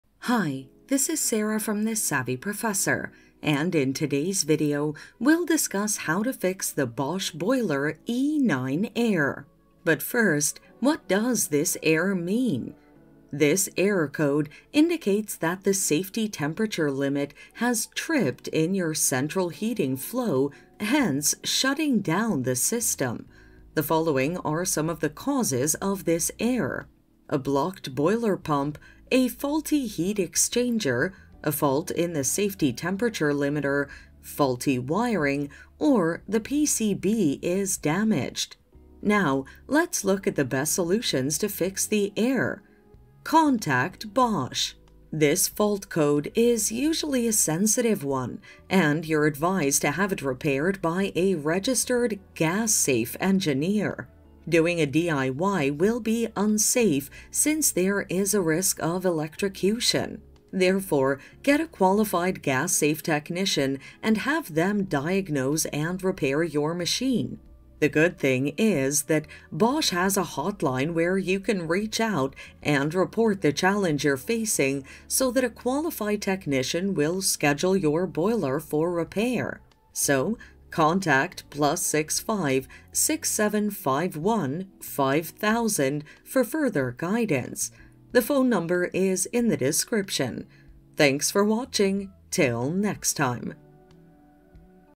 How To Fix Bosch Boiler E9 Error Code - Meaning, Causes, & Solutions (Prompt Troubleshoot!)
How To Fix Bosch Boiler E9 Error Code - Meaning, Causes, & Solutions (Prompt Troubleshoot!). In this video tutorial I will show you how to fix Bosch Boiler E9 error code.

The Bosch boiler e9 error code indicates that the safety temperature limiter has tripped in your central heating flow hence shutting down the system.
The following are some of the Causes of the Error:
● Blocked boiler pump
● Faulty heat exchanger
● Fault in the safety temperature limiter
● Faulty wiring
● PCB is damaged

Now, Let’s Look at the Best Solutions to Fix the Error:
Contact Bosch
This fault code is usually a sensitive one, and you are advised to have it repaired by a registered gas-safe engineer. Doing a DIY will be unsafe since there is a risk of electrocution. Therefore, get a qualified gas-safe technician and have them diagnose and repair your machine. The good thing is that Bosch has a hotline where you can reach out and report the challenge you are facing so that a qualified technician will schedule your boiler for repair.